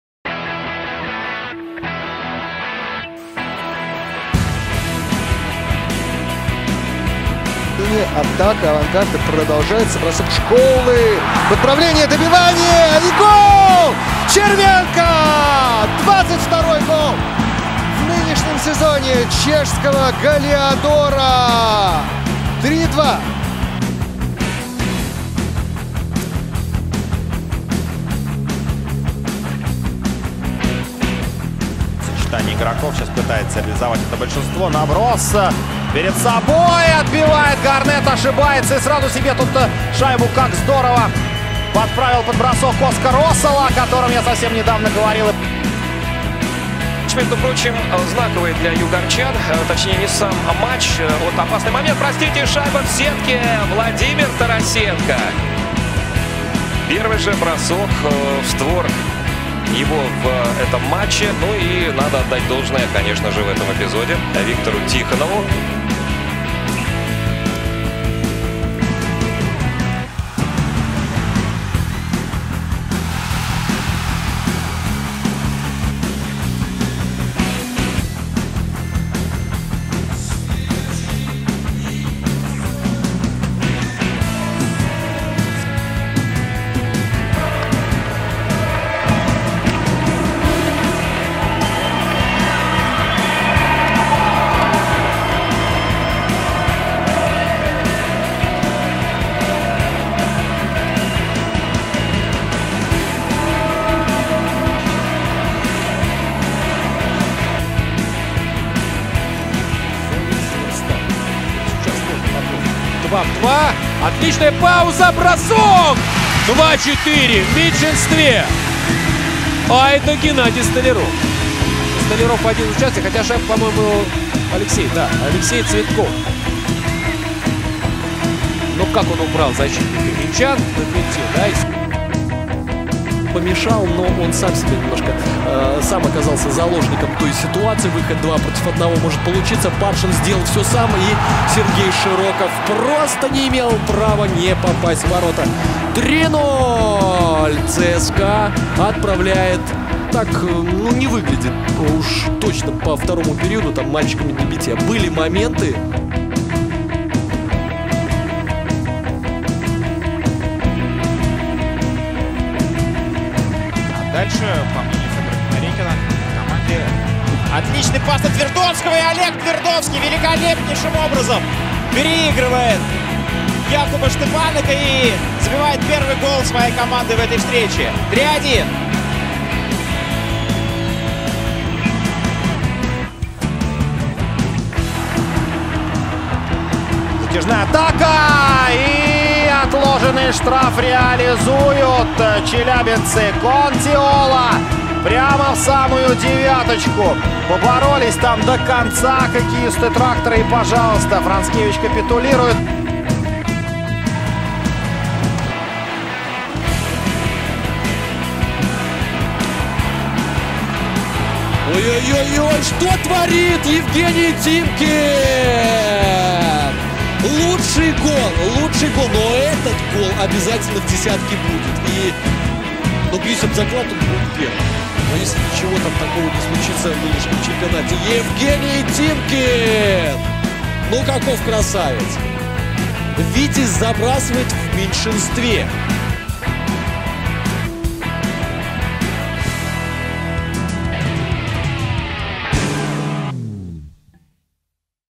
06.02 Топ-10 голов недели КХЛ / 02/06 KHL Top-1o goals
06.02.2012 Евгений Тимкин и 9 его друзей в еженедельном обзоре голов

10. Роман Червенка / Авангард
9. Оскар Осала / Нефтехимик
8. Владимир Тарасенко / СКА
7. Евгений Кетов / Северсталь
6. Микелис Редлихс / Динамо Рига
5. Алексей Цветков / Северсталь
4. Сергей Широков и Денис Паршин / ЦСКА
3. Олег Твердовский / Металлург Мг
2. Петри Контиола / Трактор
1. Евгений Тимкин / Витязь